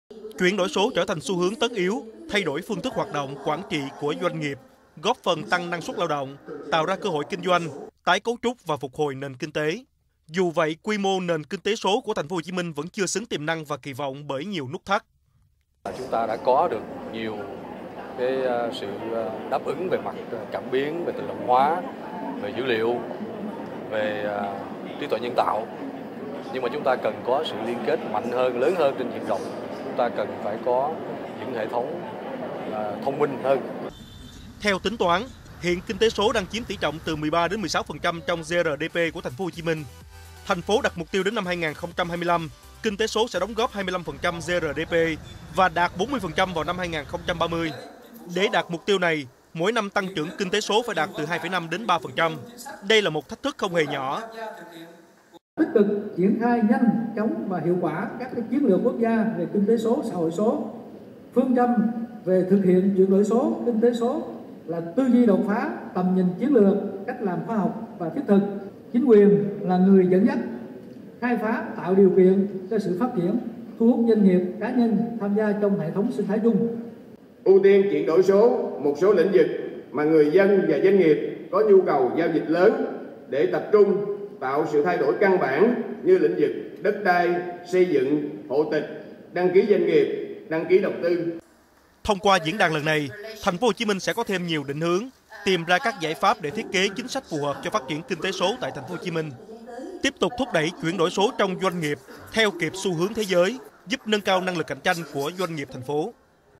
Truyền hình thông tấn - Kinh tế số sẽ là động lực tăng trưởng của TP.HCM
Chuyển đổi số trở thành xu hướng tất thay đổi phương thức hoạt động quản trị của doanh nghiệp, góp phần tăng năng suát lao động tạo ra cơ hội kinh doanh, tái cấu trúc và phục hồi nền kinh tế. Dù vậy quy mô nền kinh tế số của Thành phố Hồ Chí Minh vẫn chưa xứng tiềm năng và kỳ vọng bởi nhiều nút thắt.
Ông Lê Công Tuấn Kiệt, Phó Tổng Giám đốc Công ty Cổ phần Bóng đèn Điện Quang cho biết: "Chúng ta đã có nhiều sự đáp ứng về mặt cảm biến, tự động hóa về dữ liệu, về trí tuệ nhân tạo, nhưng chúng ta cần có sự liên kết mạnh hơn, lớn hơn trên diện rộng, chúng ta cần phải có những hệ thông minh hơn."
Nguồn: Truyền hình thông tấn